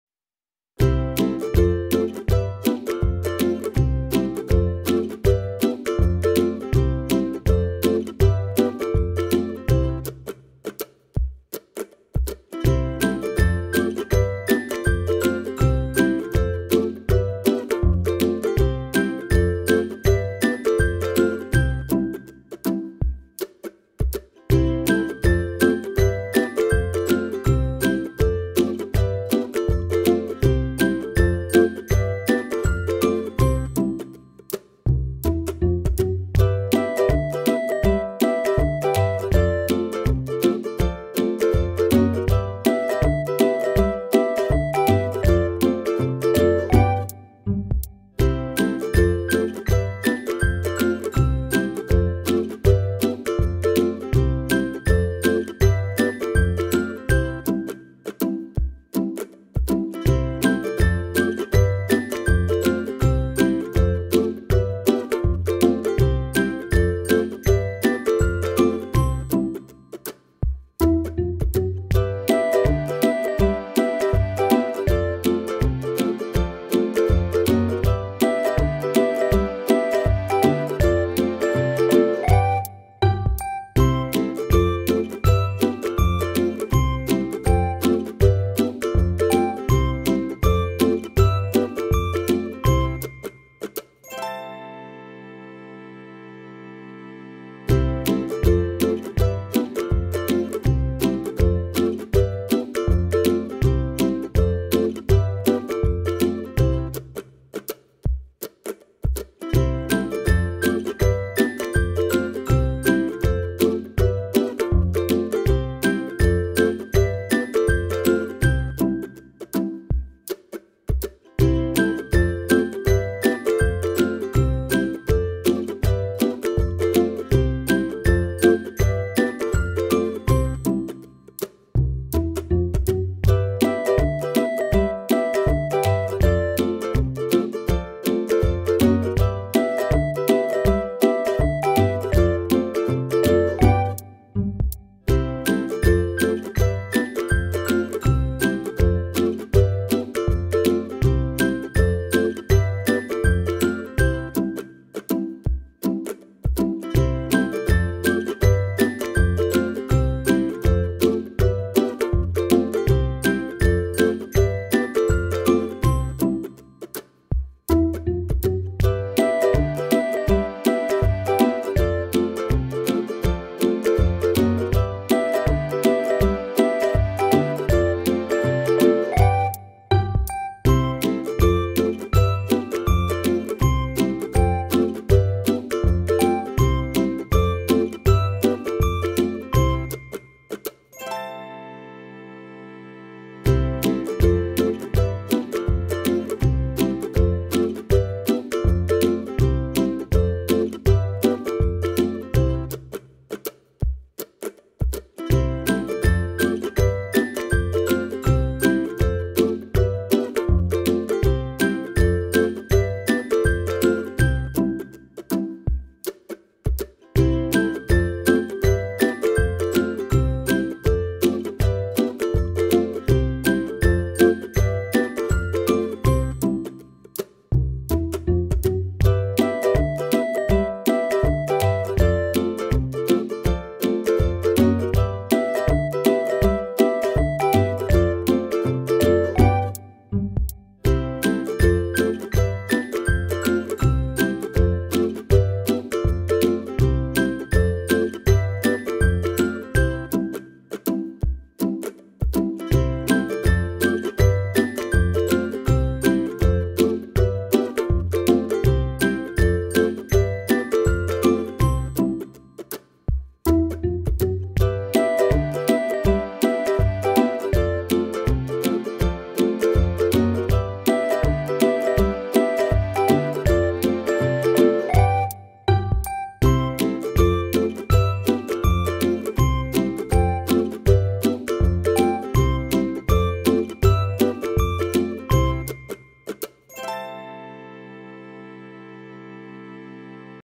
Kerajinan bunga dari tutup botol // Hiasan meja dari tutup botol // Flower from bottle cap
Memanfaatkan tutup botol jadi hiasan cantik yuk.
bahannya hanya tutup botol dan manik-manik. yuk kepoin videonya